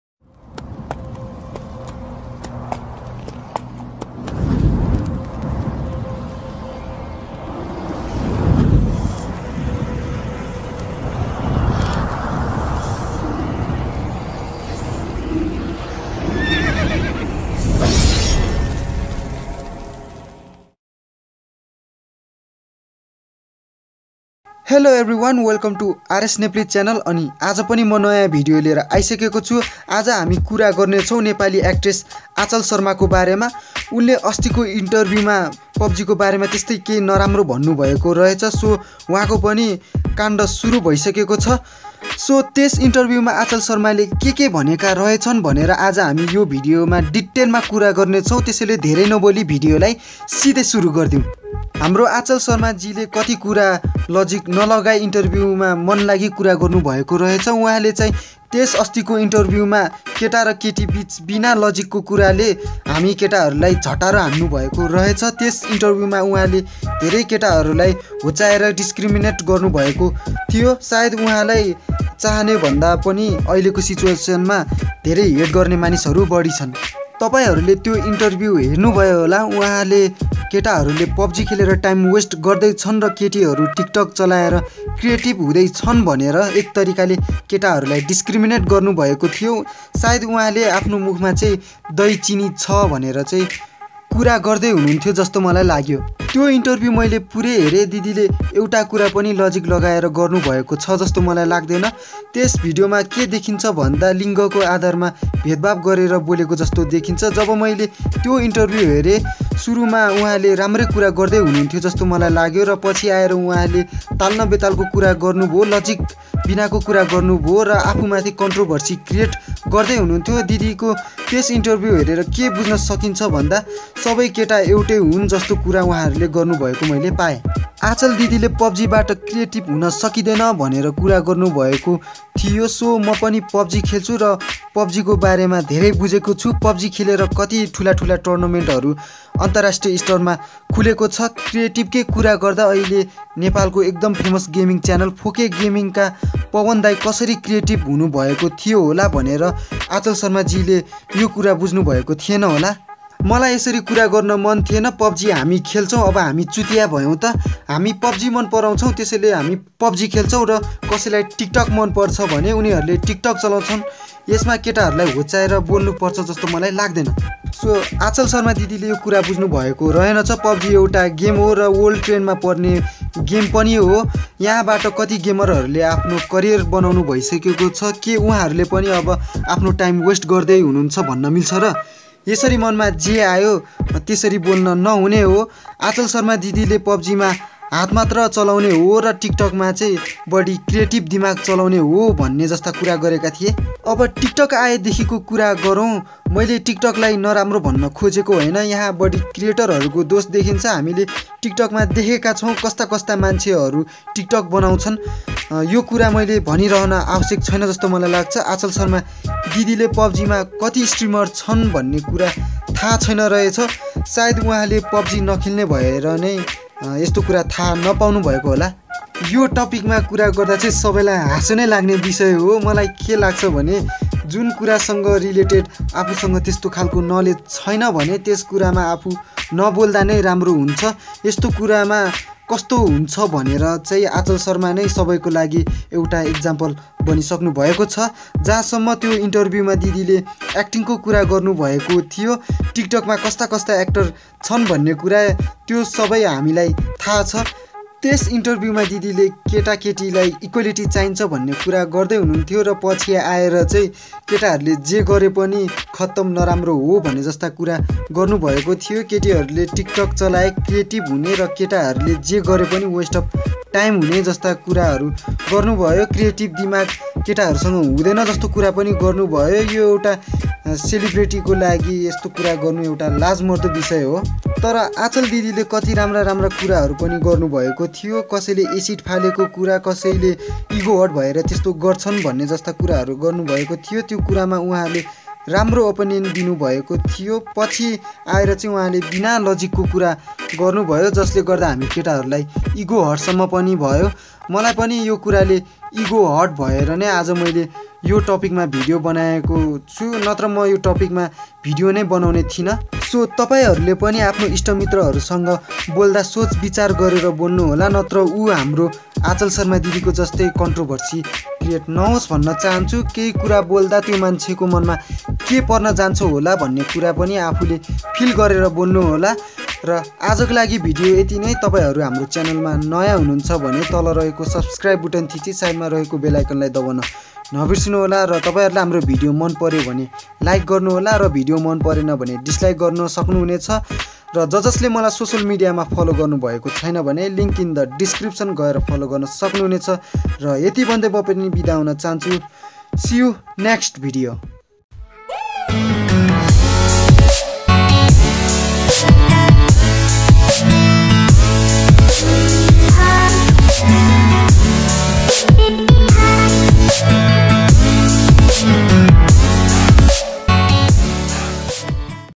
Aanchal sharma talk about pubg| Tiktoker are more creative than pubg player| Pubg vs Tiktok | Ep-23
If you have any problem in this video, you can message us under social media.

Keep loving and keep supporting us.